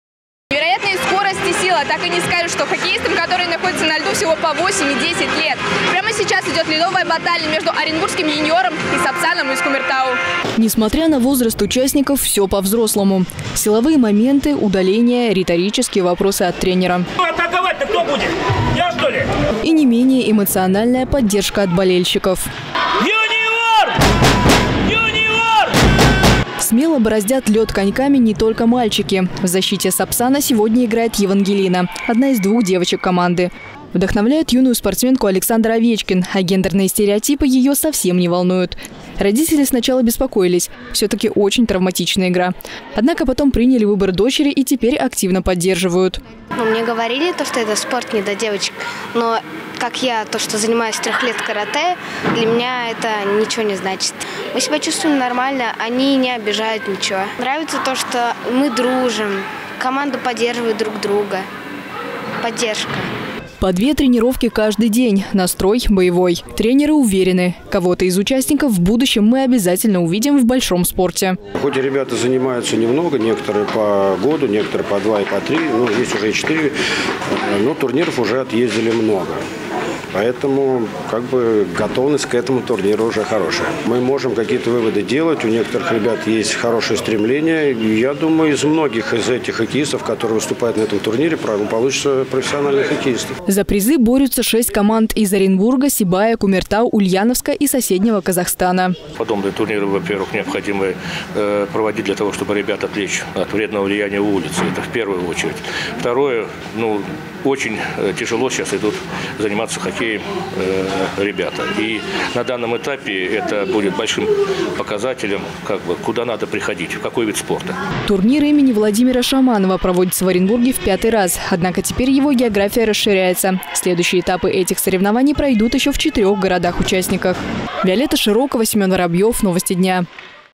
Всероссийский день хоккея
Трус не играет в хоккей. 1 декабря отмечается Всероссийский праздник традиционного зимнего вида спорта. Он утвержден в 2007 году Федерацией хоккея страны. С тех пор в этот день проводятся товарищеские матчи. Юные хоккеисты из Оренбурга и Кумертау померились силами.